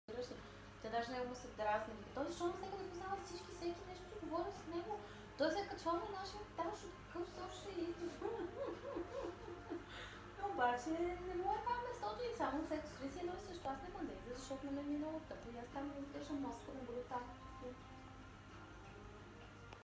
Oscar Fish group
11 бр. от 6-7 до 14 см. Тигрови, дива форма и един албино